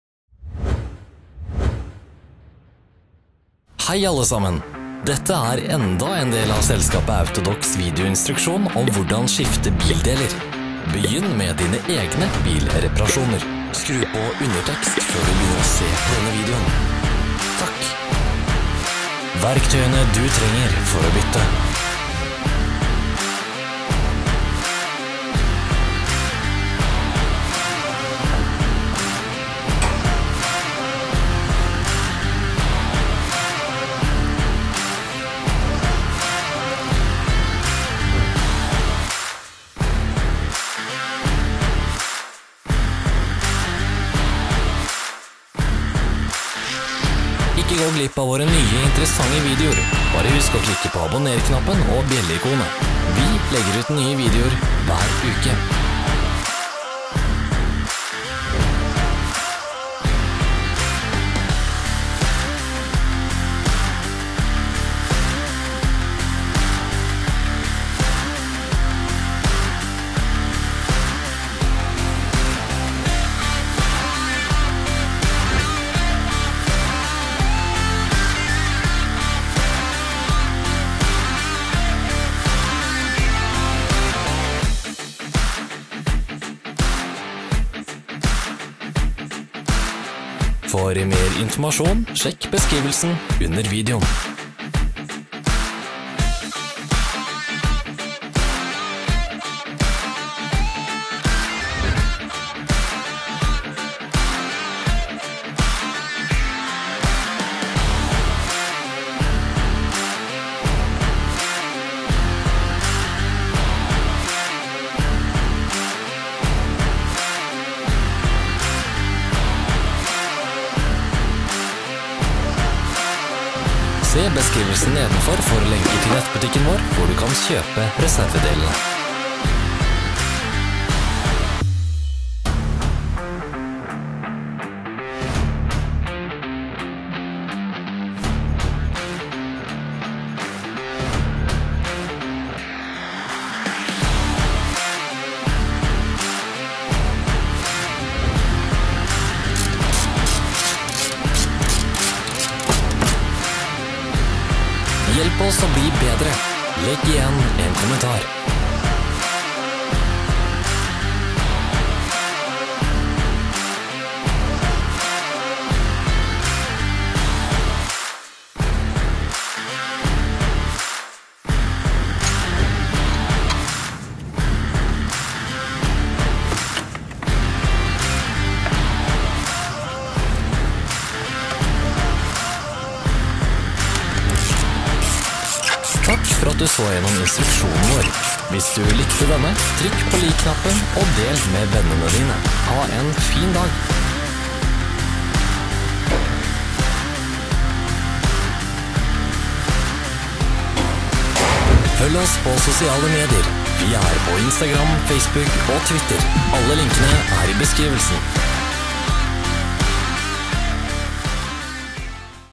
Hvordan bytte multireim / kileribberem der på TOYOTA PRIUS 2 (NHW20) [AUTODOC-VIDEOLEKSJONER]
Hvordan bytte multireim / kileribberem på TOYOTA PRIUS 2 (NHW20) 1.5 Hatchback 08.2003–03.2009 [AUTODOC-VIDEOLEKSJONER]

Multireim for kjøretøyet TOYOTA Prius II Liftback (XW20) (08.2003–03.2009) bør som regel erstattes i én og samme prosedyre. Det er imidlertid noen små forskjeller, på grunn av spesifikke egenskaper ved kjøretøyets design.

🔝 De beste merkene som produserer multireim: BOSCH, CONTITECH, GATES, SKF, DAYCO, INA, STARK.

Alle video- og PDF-veiledninger om utskifting av deler i TOYOTA PRIUS 2: last ned og se på AUTODOC CLUB

🛠️ Verktøyene du trenger for å erstatte multireim:
- kombinøkkel nr. 12
- pipenøkler nr.10
- pipenøkler nr.12
- pipenøkler nr.14
- skrallenøkkel
- phillips-skrutrekker
- skjermtrekk
- stoppekloss